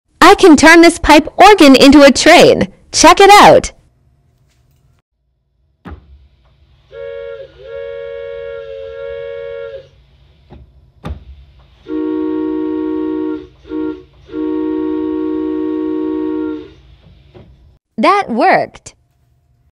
Turning this small pipe organ into a train.
Never done this sound on a pipe organ before, so I would give it a try.